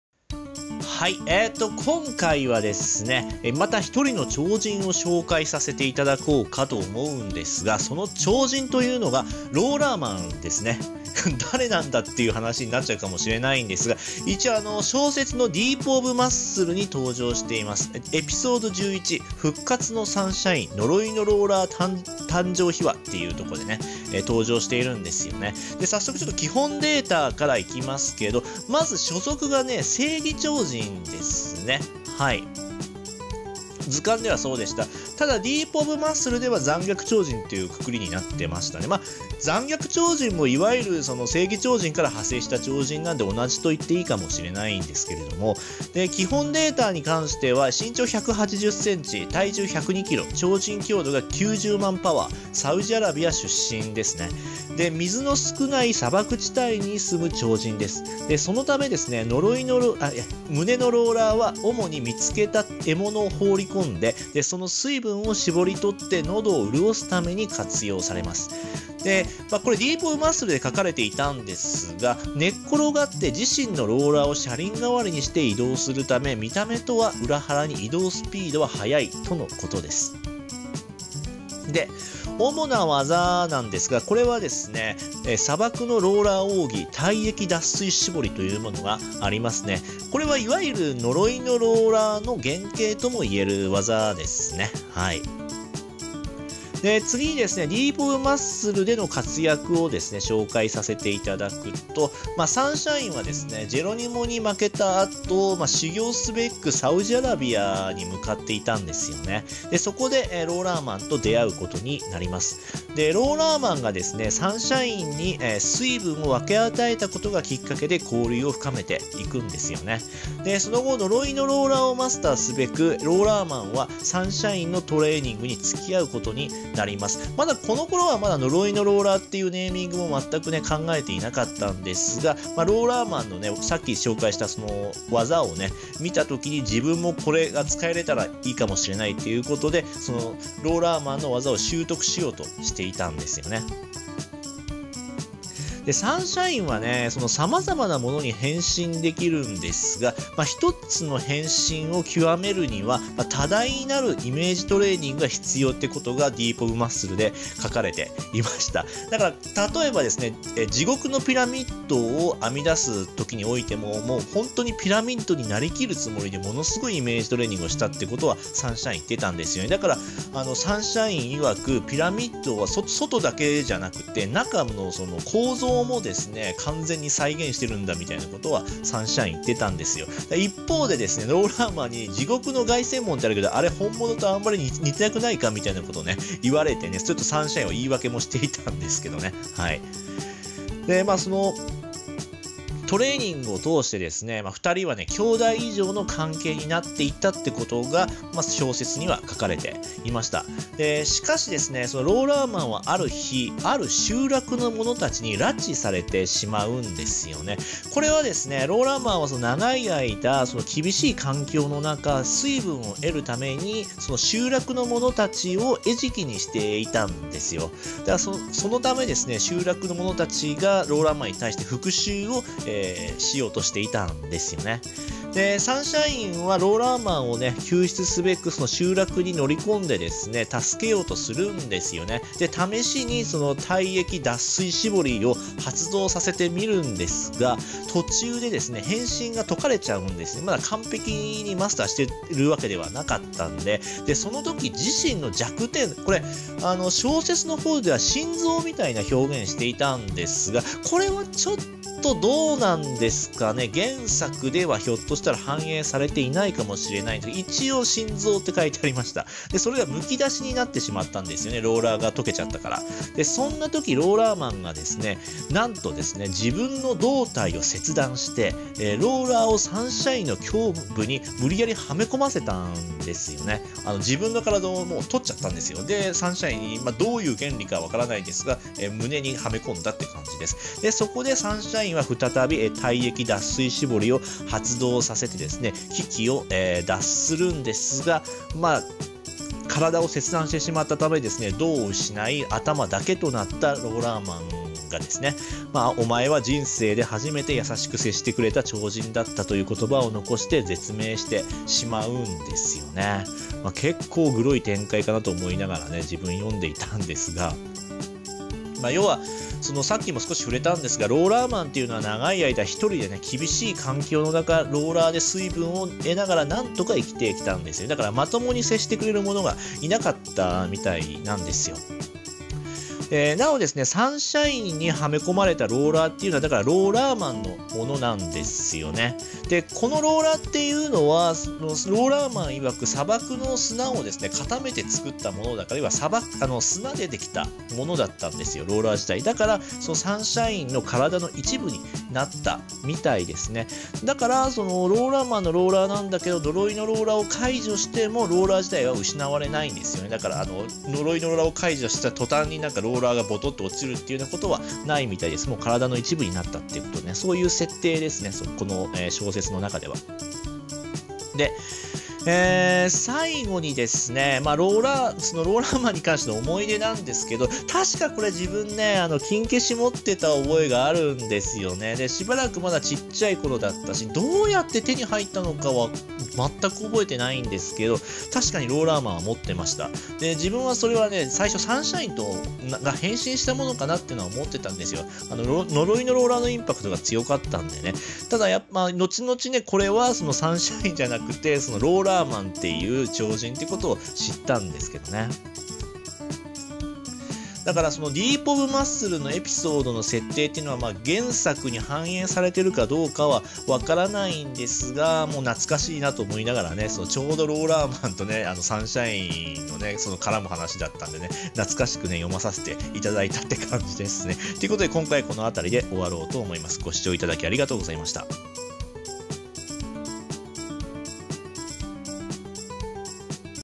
【キン肉マン/ストーリー考察・予想#203】ローラーマンを紹介！【呪いのローラー誕生秘話】
※キン肉マン、キン肉マンⅡ世のネタバレ含みますのでご注意ください。
☆twitter始めました！

☆画像引用元

キン肉マン マッスルショット 最速攻略wiki

☆音源引用元